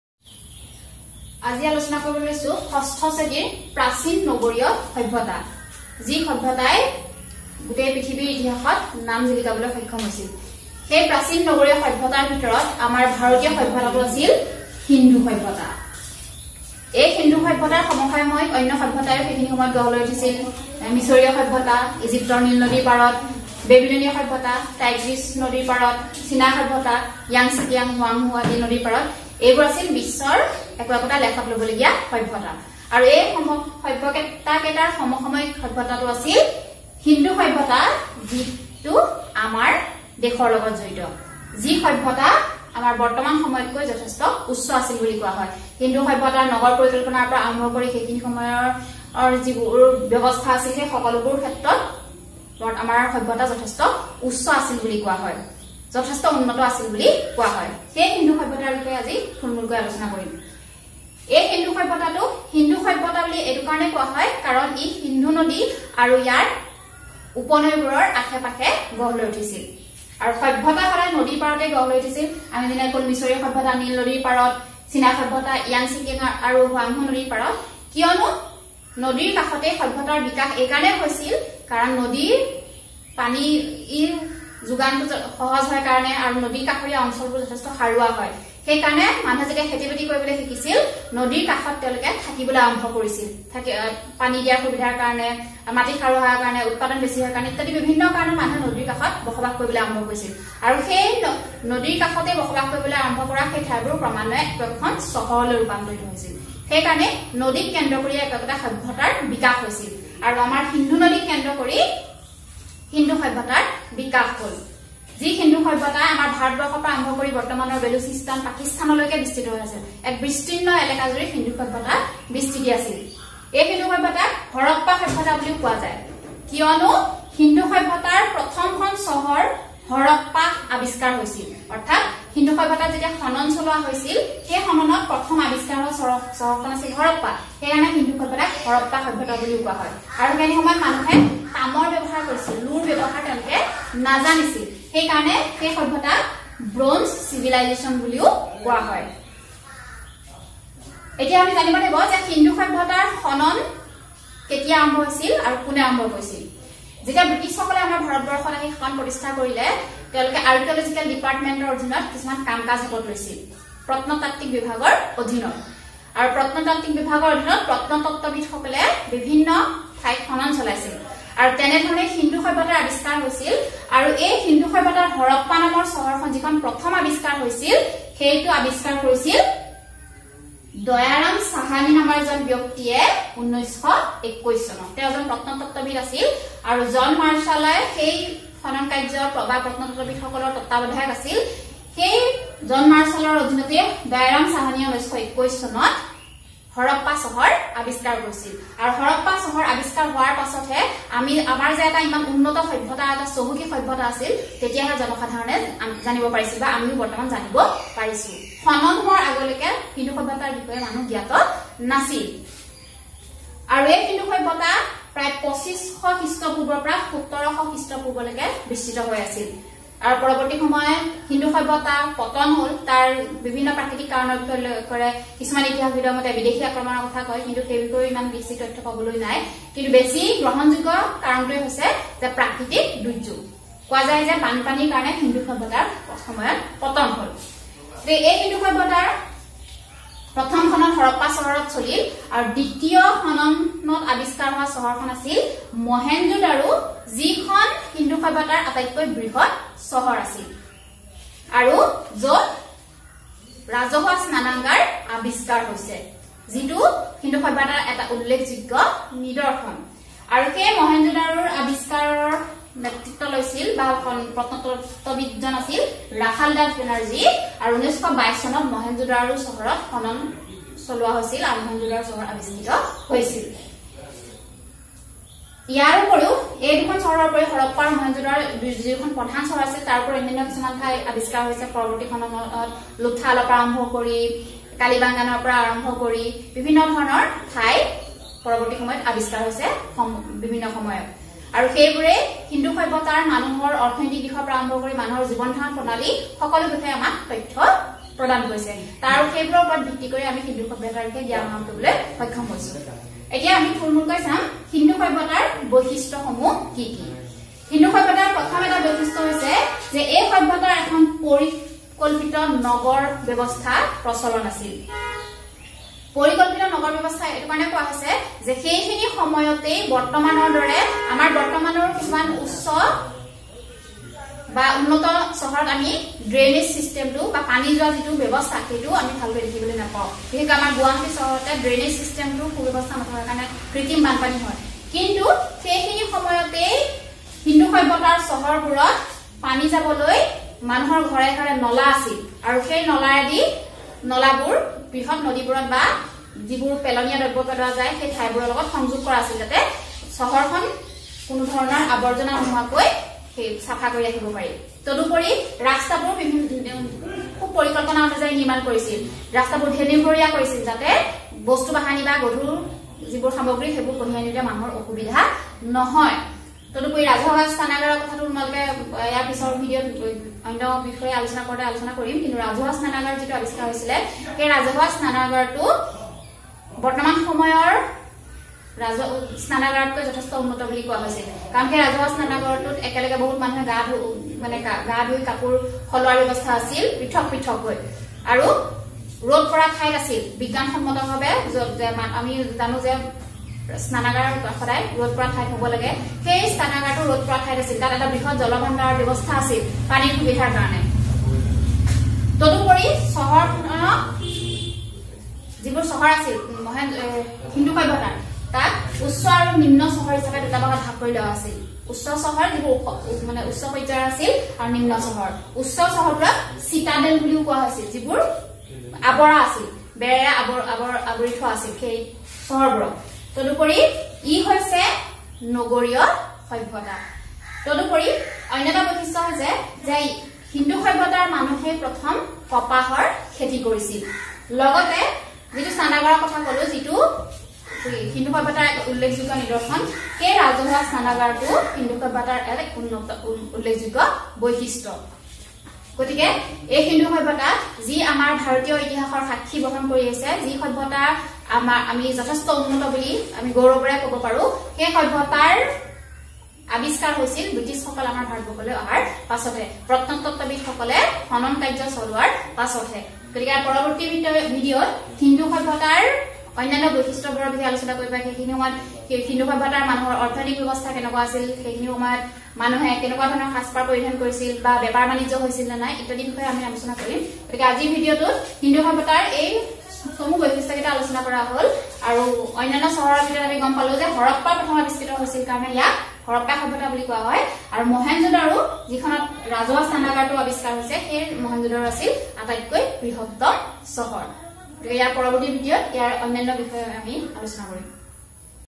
Class 6 Social Science Chapter 12(SEBA Assamese Medium)
It's about the basic features of the Indus Civilization,chapter "Ancient Urban Civilization".